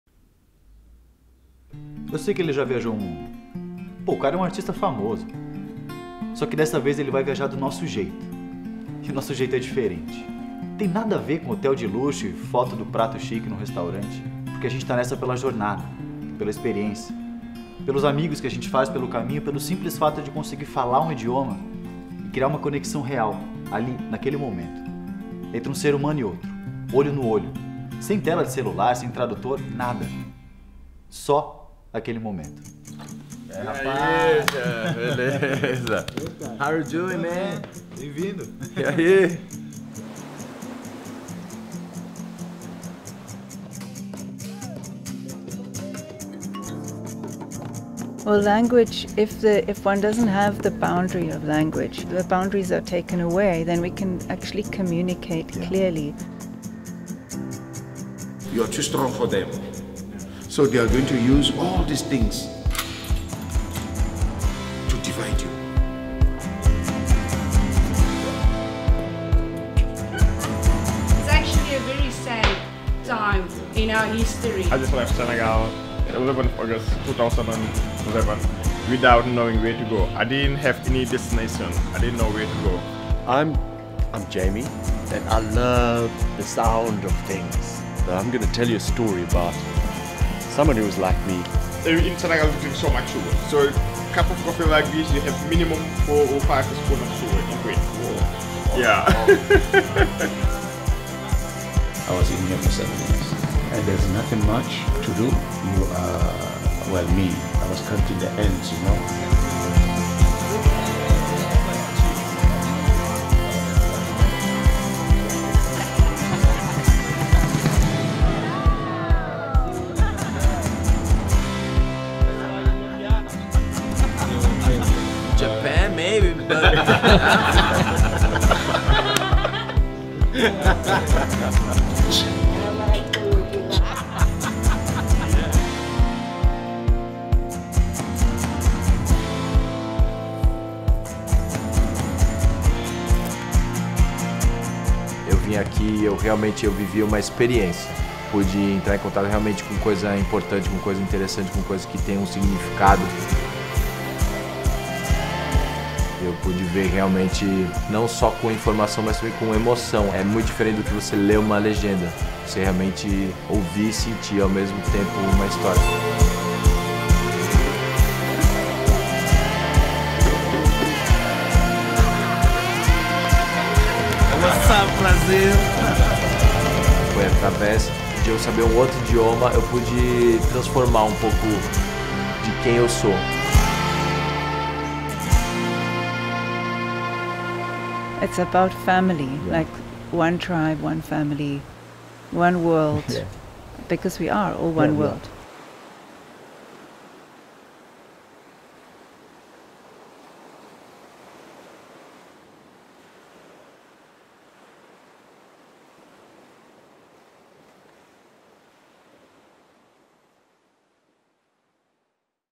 KNN On the Road with Marco Luque @ South Africa
Encontros nos transformam e por isso, queremos espalhar essa mensagem de união. Um só mundo. Ubuntu.
A KNN e o @marcoluque foram para a África do Sul no mais recente On the Road. E através das conexões que o idioma nos proporcionou, conhecemos mais intimamente as histórias e culturas de um país incrível.